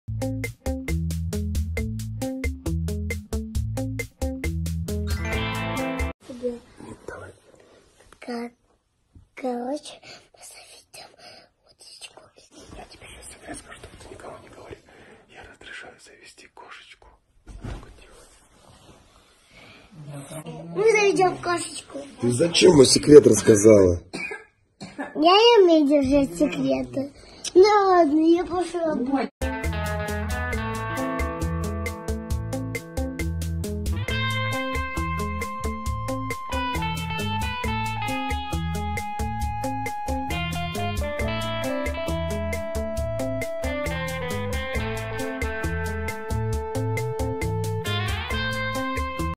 Бородина запретила, а Курбан разрешил Теоне купить кошку (ondom2.com)
В этом выпуске новости и слухи  про Ксению Бородину и её мужа Курбана Омарова.